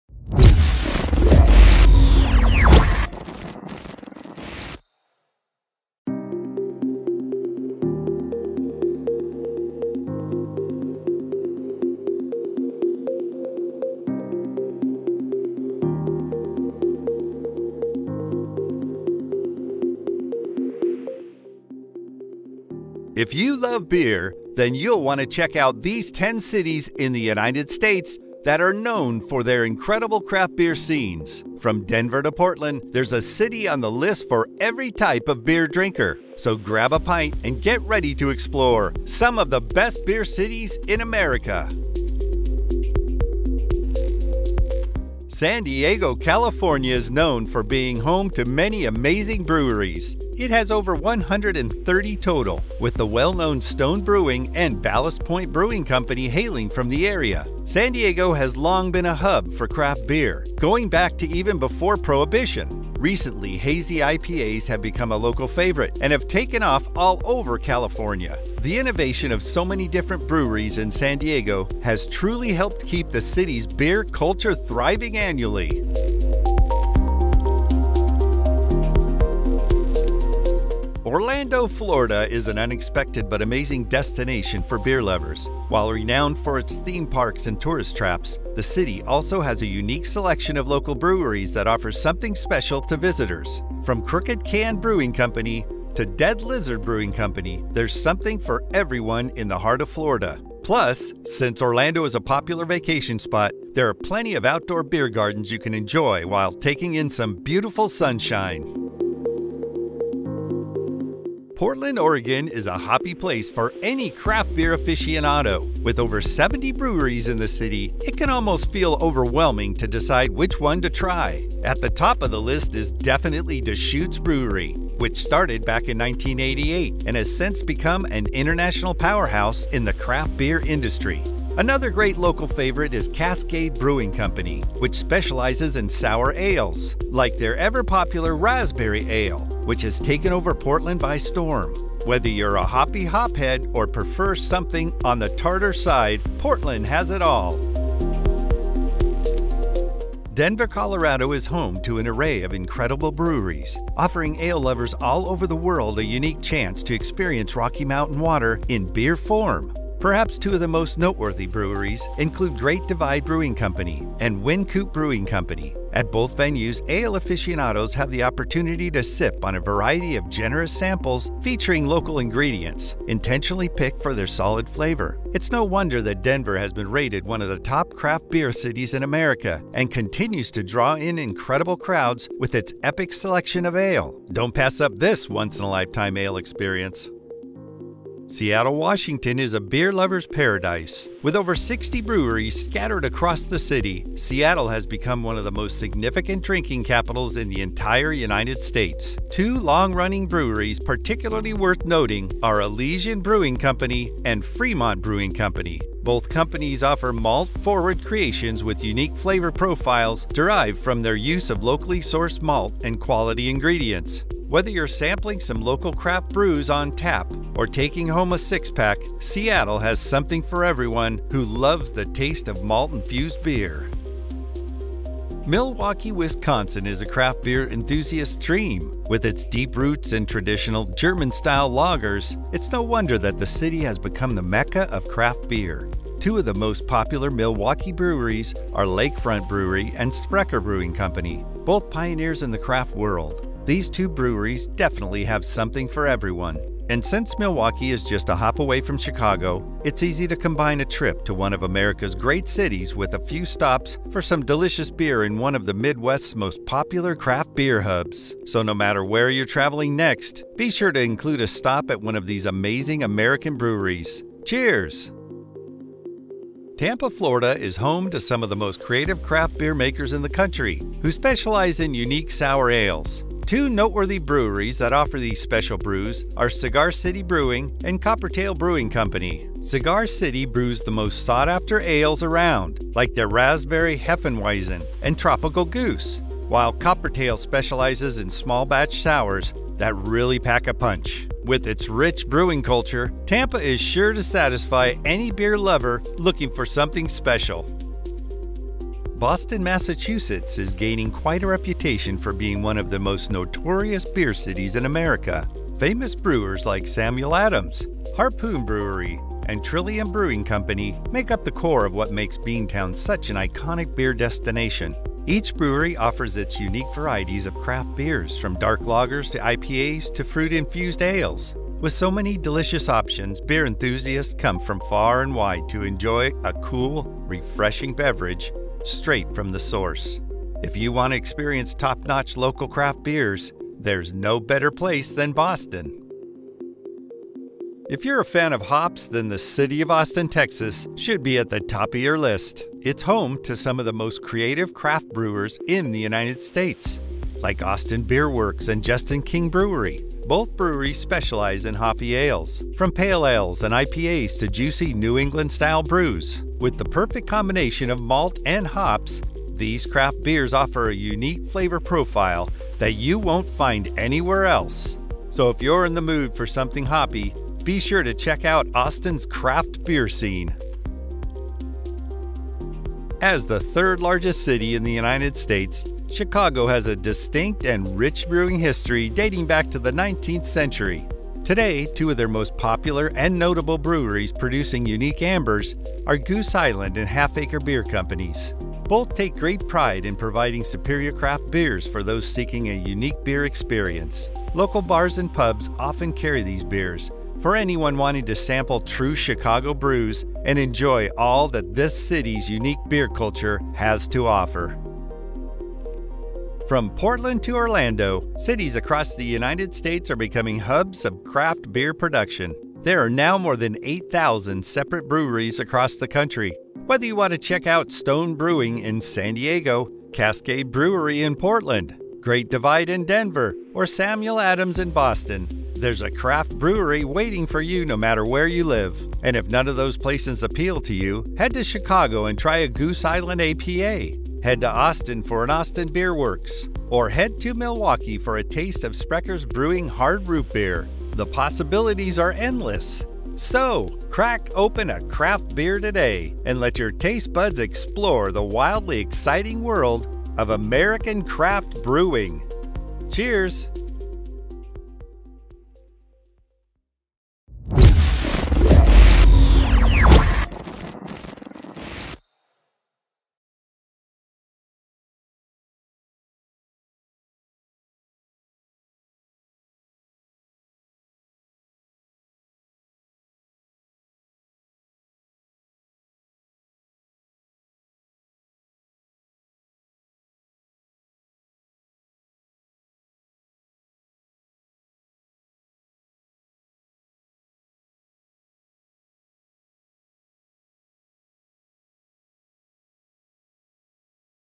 10 Best Beer Cities in the United States Part I
If you love beer, then you'll want to check out these 10 cities in the United States that are known for their incredible craft beer scenes. From Denver to Portland, there's a city on this list for every type of beer drinker. So grab a pint and get ready to explore some of the best beer cities in America!